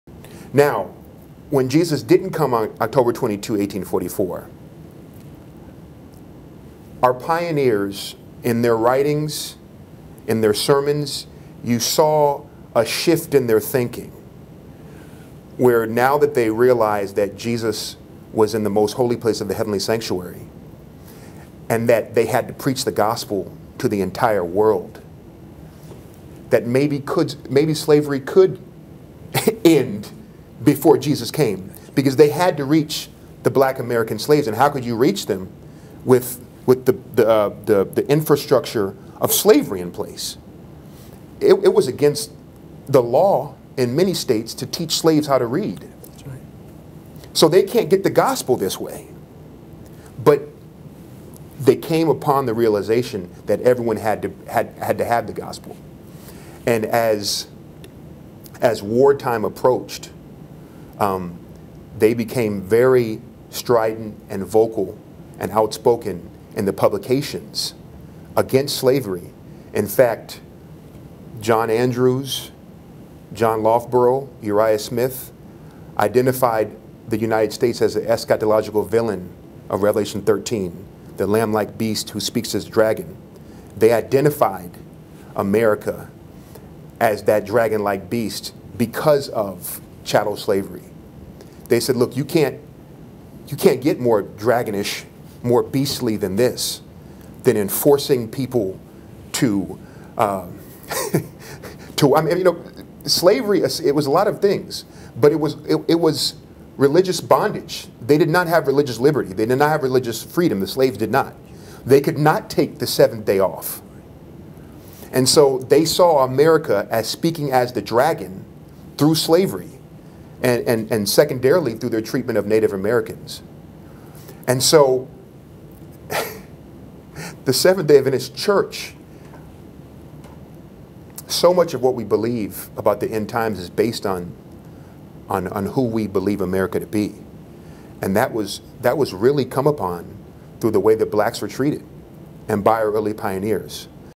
Slavery and America in Revelation 13 - Dr. Benjamin Baker
Dr. Benjamin Baker speaks on how Adventist pioneers used slavery to identify the lamblike beast of Revelation 13 (Interview at the World Headquarters of Seventh-day Adventists on February 19, 2015)

This video was produced by the Media Ministries of Allegheny East Conference of Seventh-day Adventists